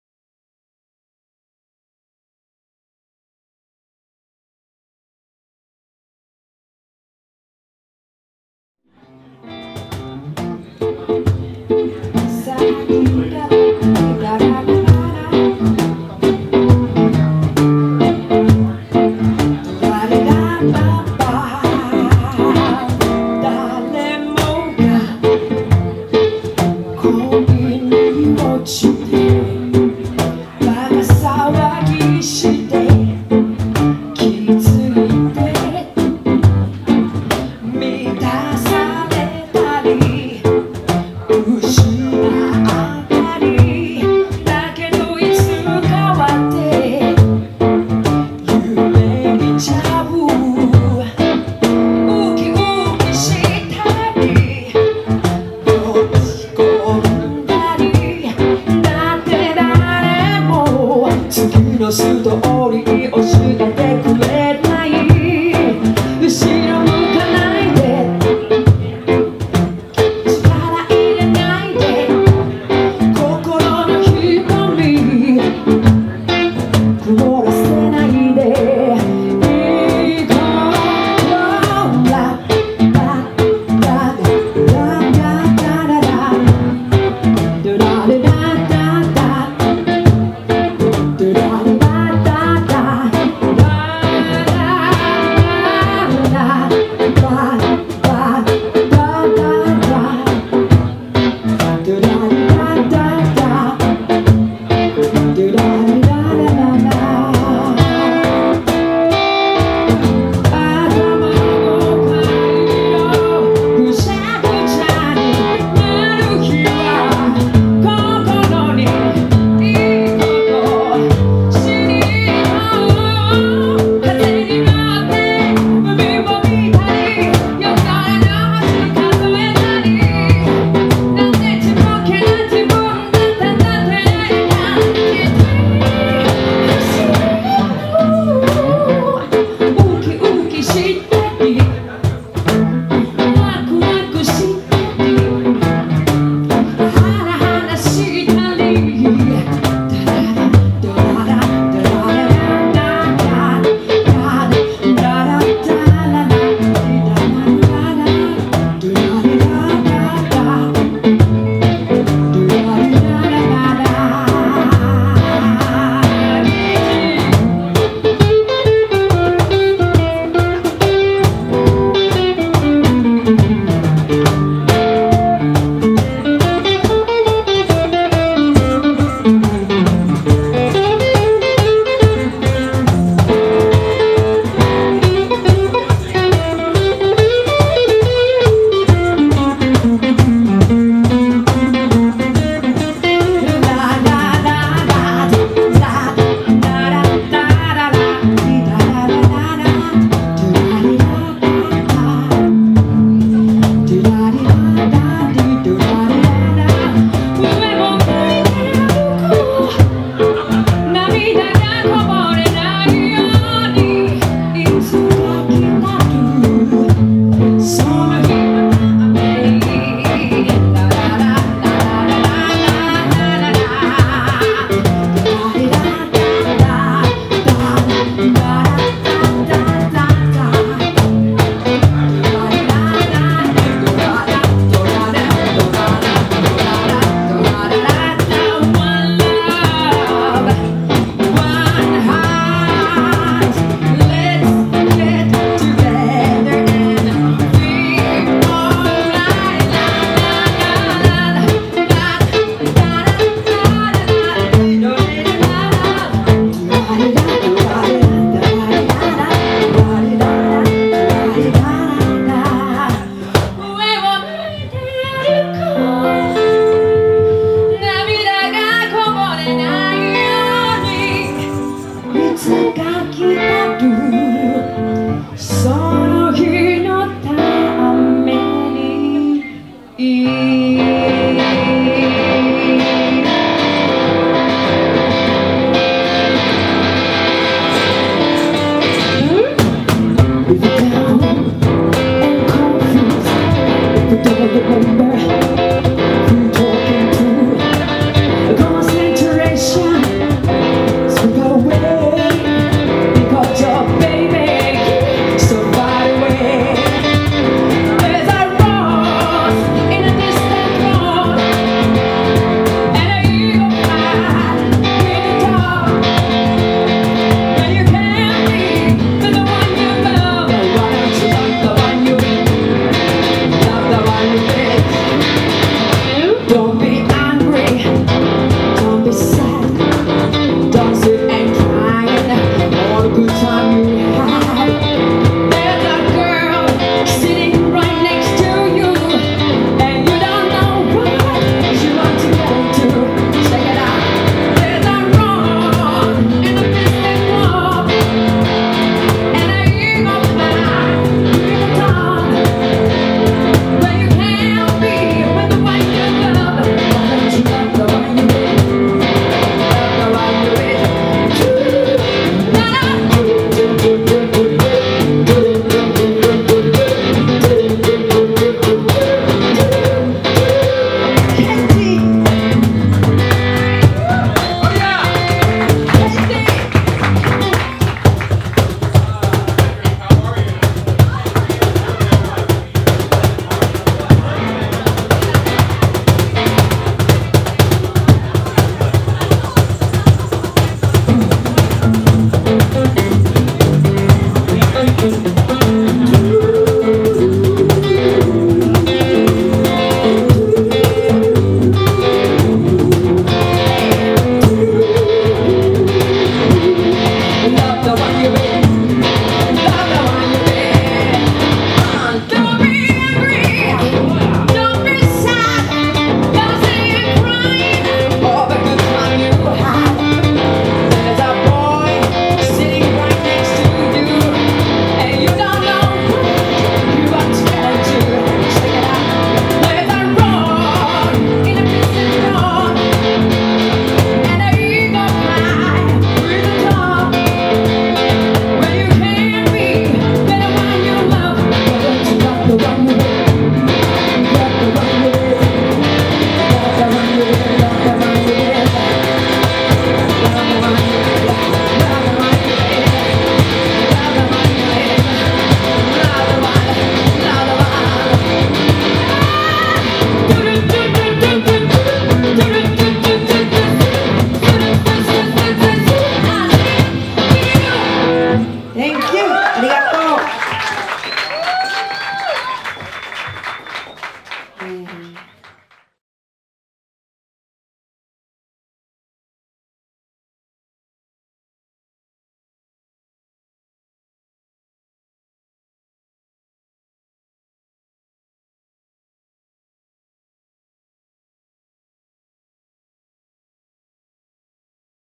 naomi hayashi. j-summit earthquake and tsunami relief fund vol.12.wmv
artists and performers doing their thing for continued help for the japan earthquake and tsunami relief. this is naomi hayashi performing december 23, 2012 at the bowery electric in nyc.